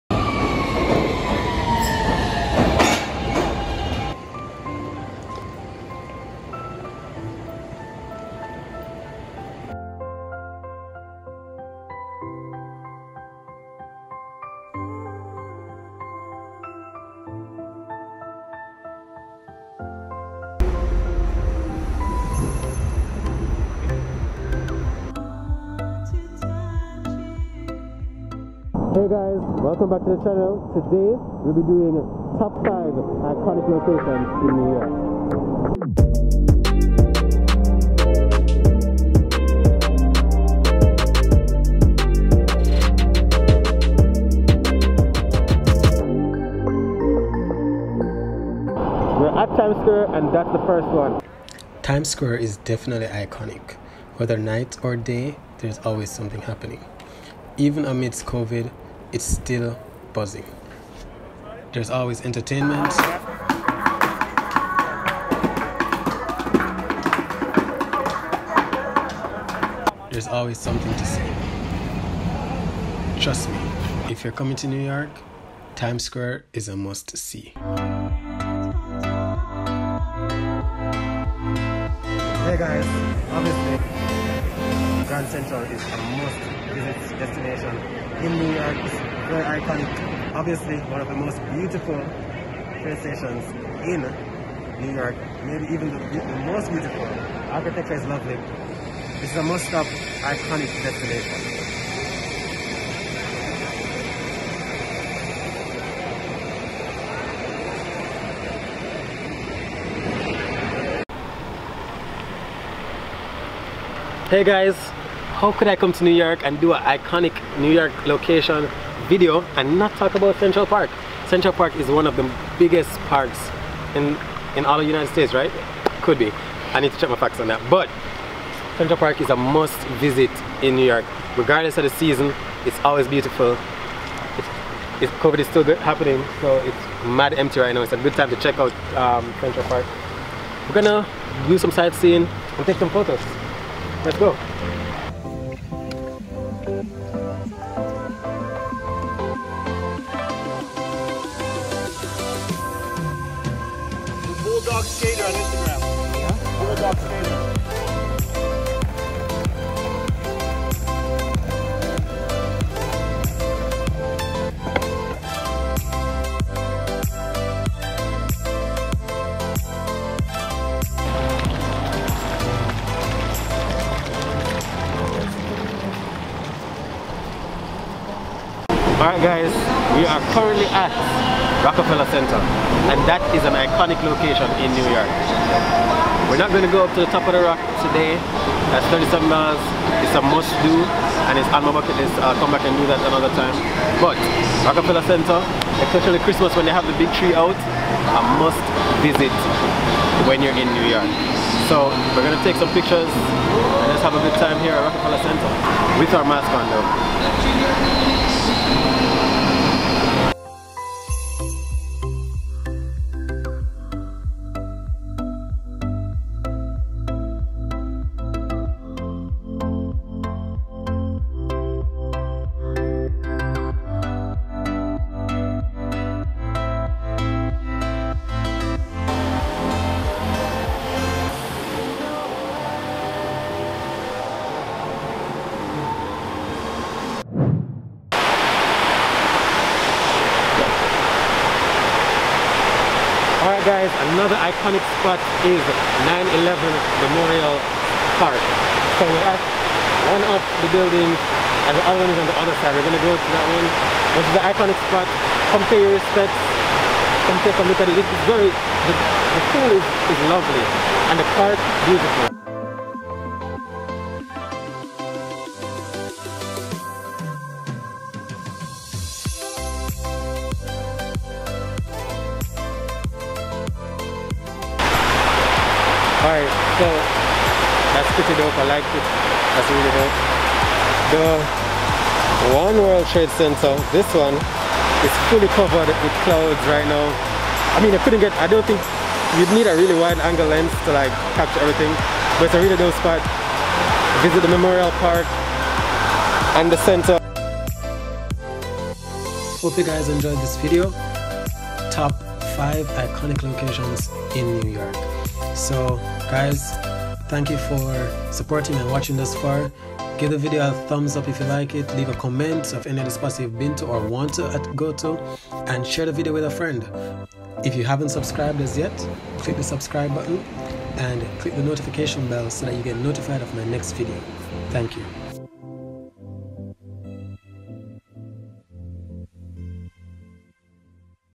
5 Iconic NEW YORK Locations | NY During Covid 2020
5 Iconic NEW YORK Locations
New York during Covid Oct 2020

Some Well known locations that you should visit while it's less crowded.

Teespring store:
White:-